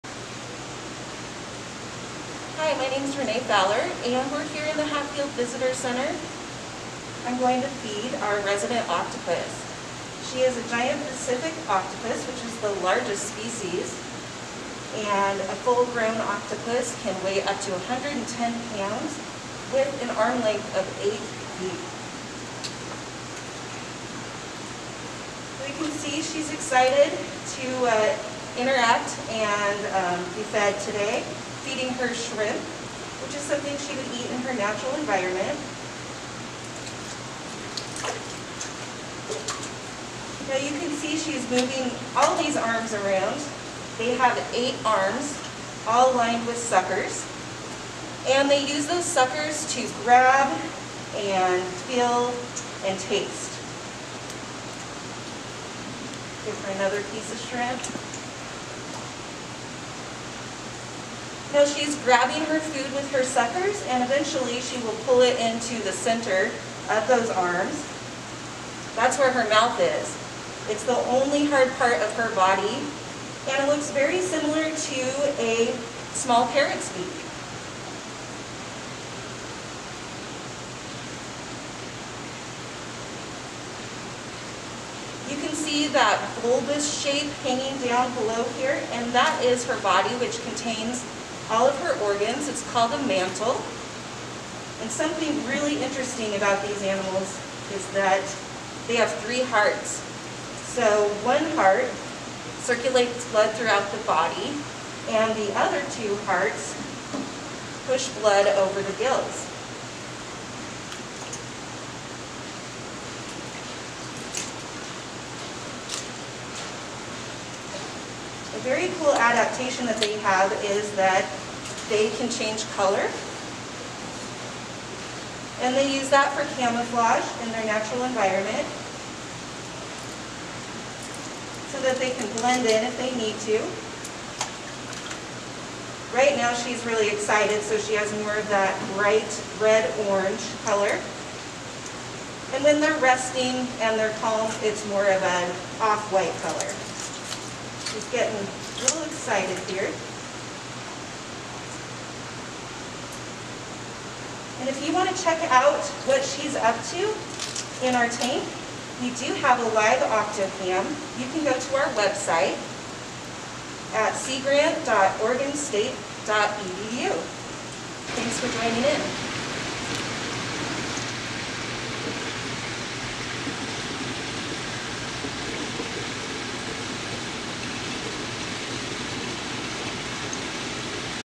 Octopus feeding at Hatfield Visitor Center in Newport
Oregon Sea Grant's Renee Fowler feeds the giant Pacific octopus at Oregon Sea Grant's Visitor Center at the Hatfield Marine Science Center in Newport.